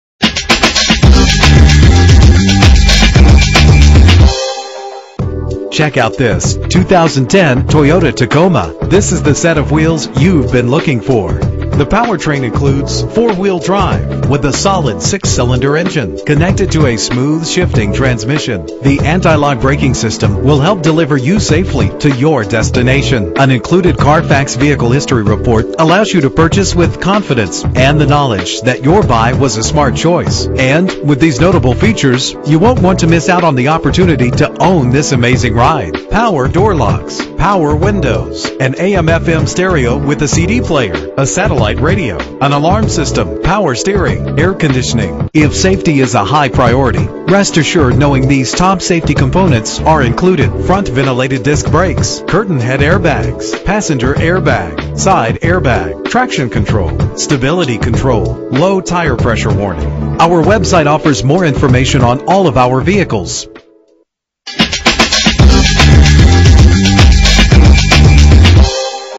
2010 Toyota Tacoma - Daphne AL
Year: 2010
Make: Toyota
Model: Tacoma
Trim: Base
Engine: 4.0 liter 6 cylinder 24 valve
Transmission:
Color: Silver
Mileage: 39061
Address:
27161 Hwy. 98